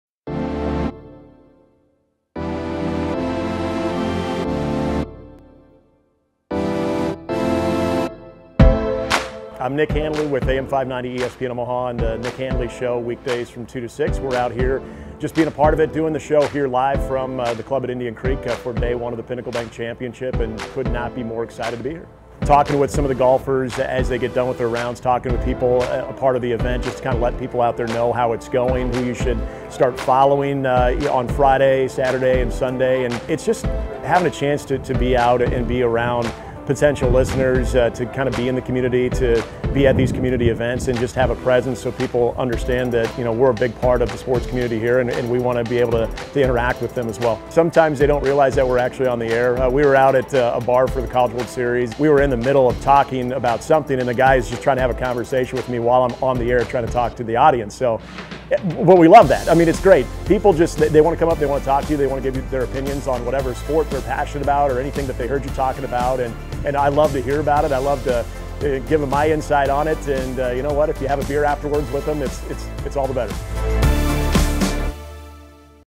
Nick Handley of ESPN 590 | Pinnacle Bank Championship
Special thanks to Nick Handley and ESPN 590 for joining us at the 2019 Pinnacle Bank Championship!